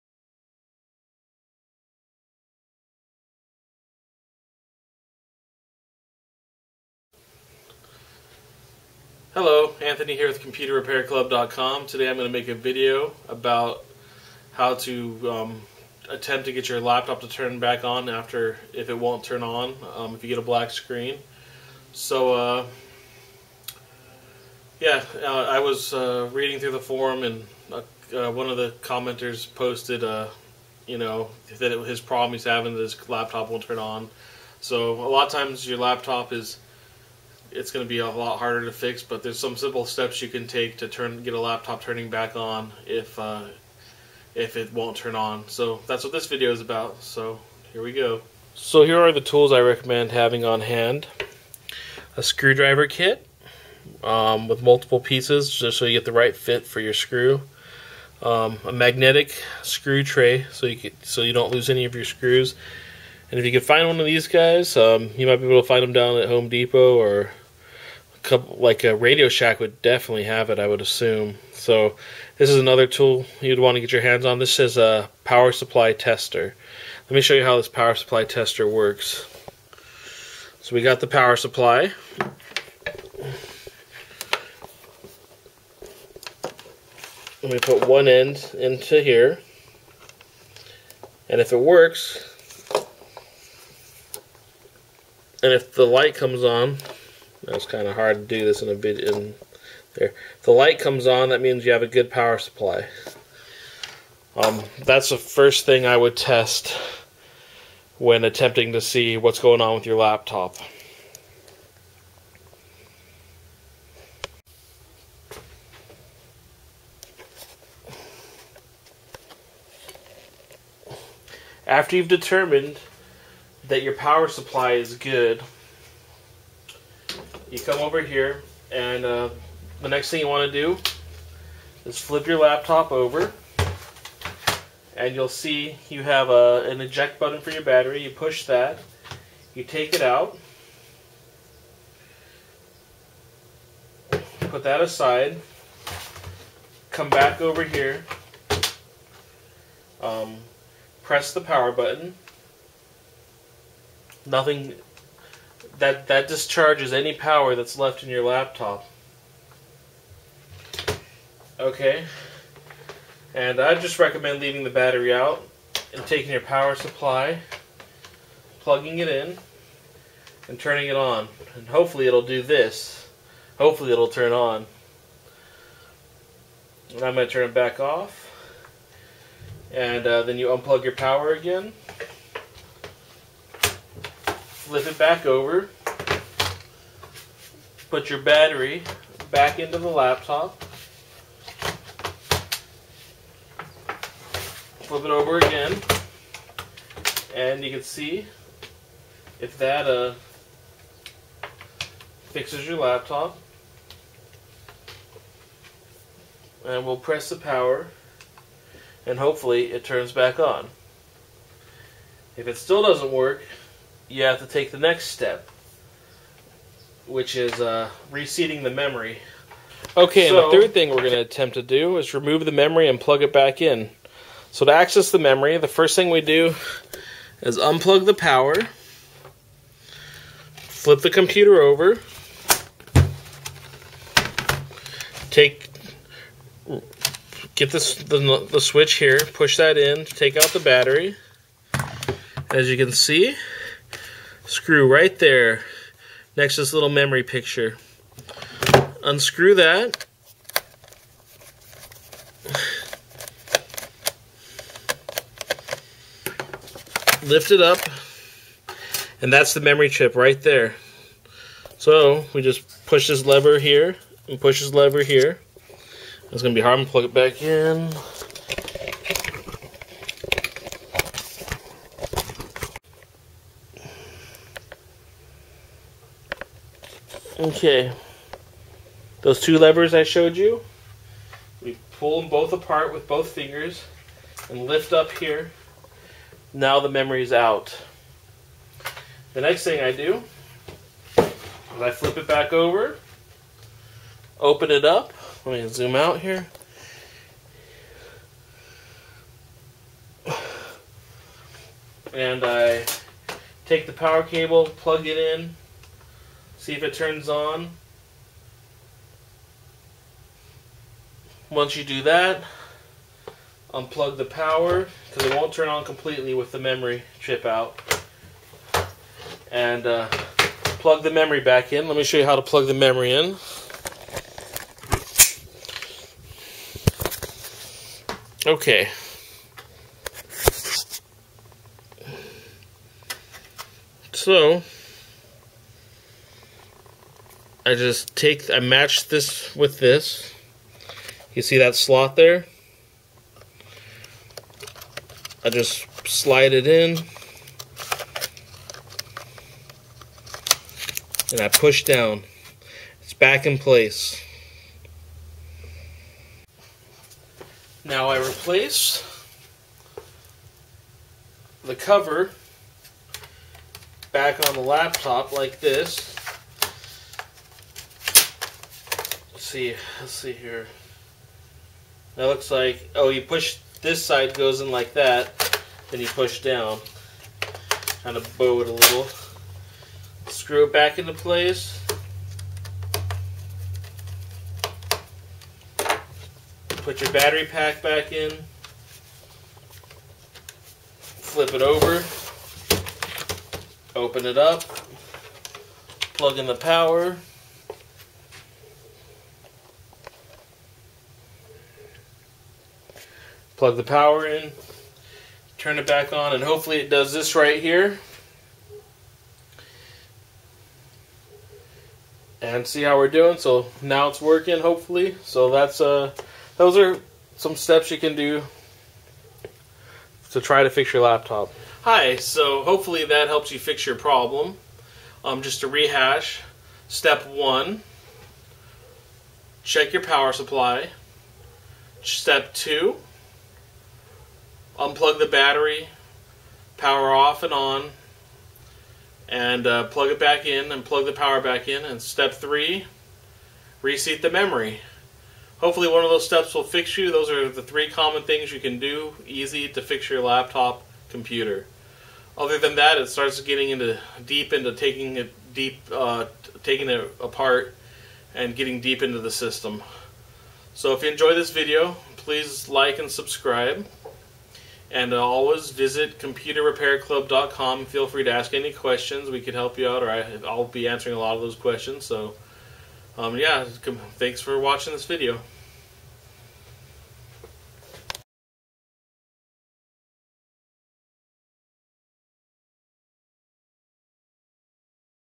Quick things to do to fix a laptop that wont turn on - Compaq CQ57
In this video i go through 3 steps you can try to fix a laptop computer that wont turn on.

The power supply tester can be purchased on ebay or radio shack.